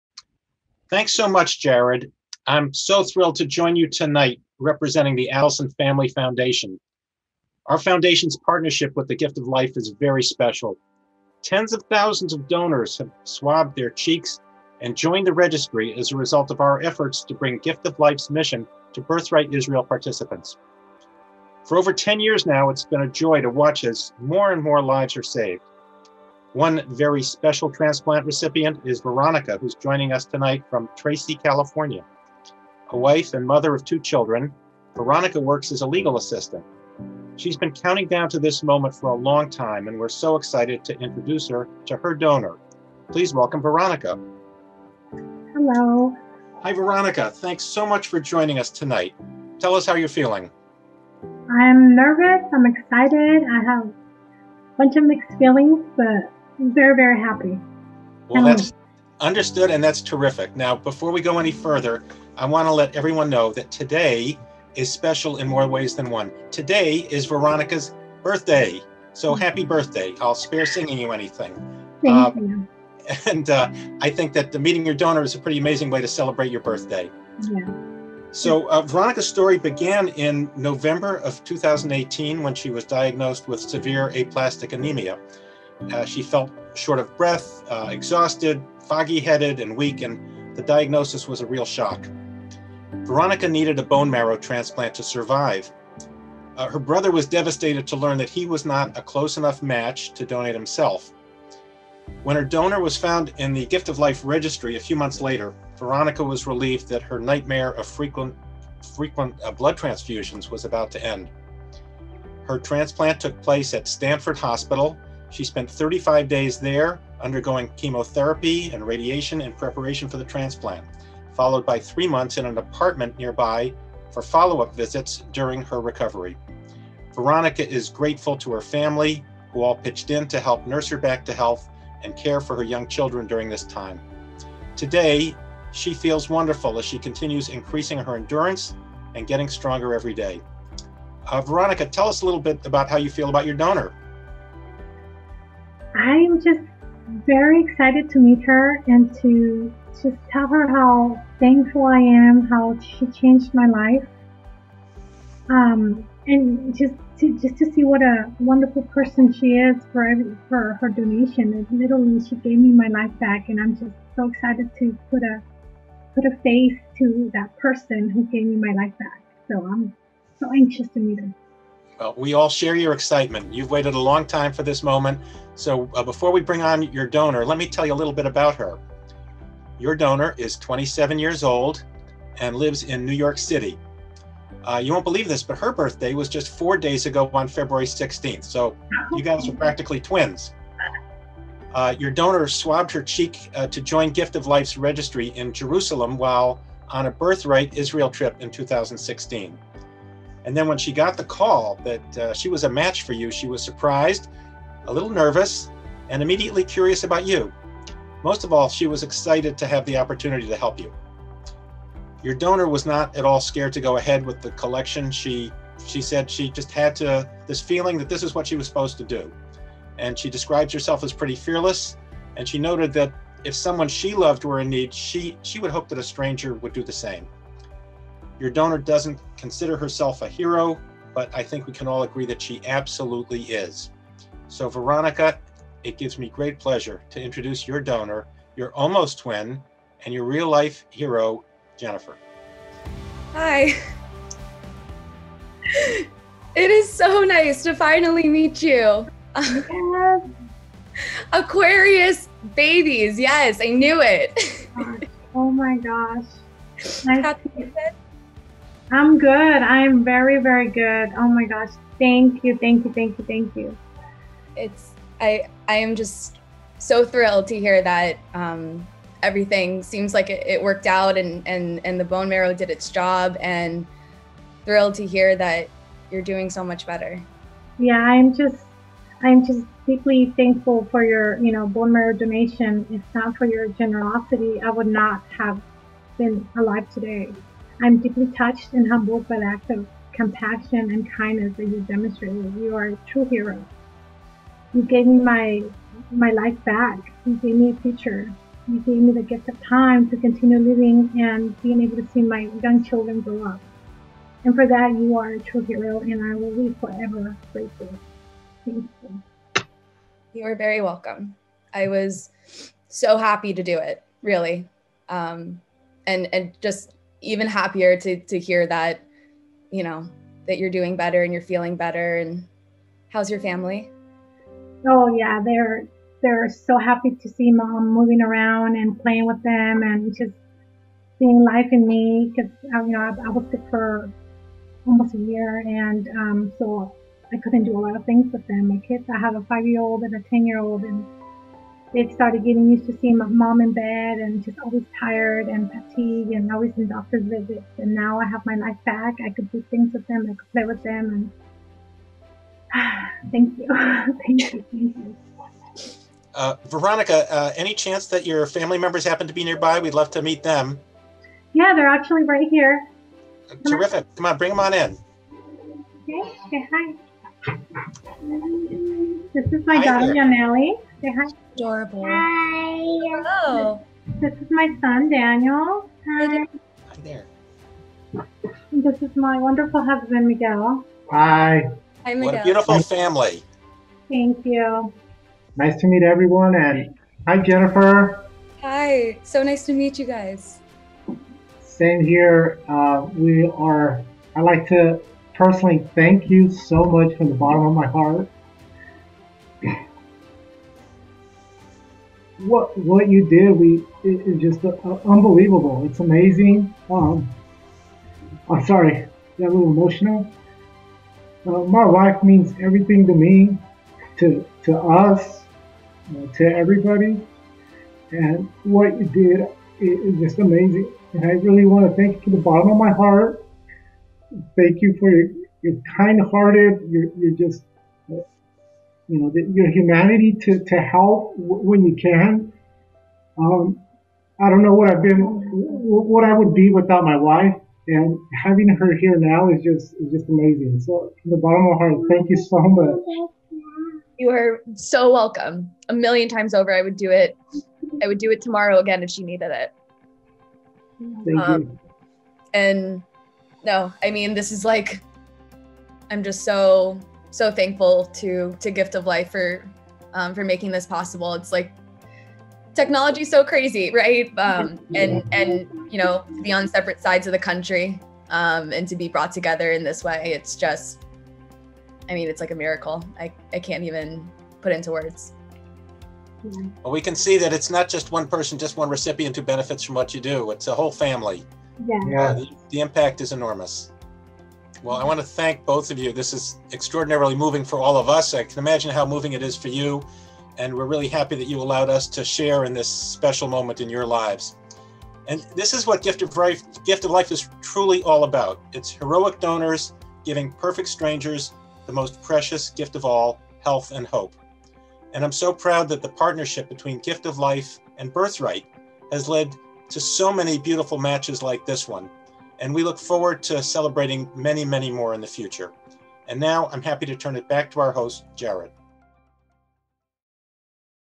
2021 Virtual Celebrating Life donor/recipient meeting
Marrow donor Jennifer Winick met her transplant recipient Veronica Ruiz virtually for the first time on Feb. 20, 2021.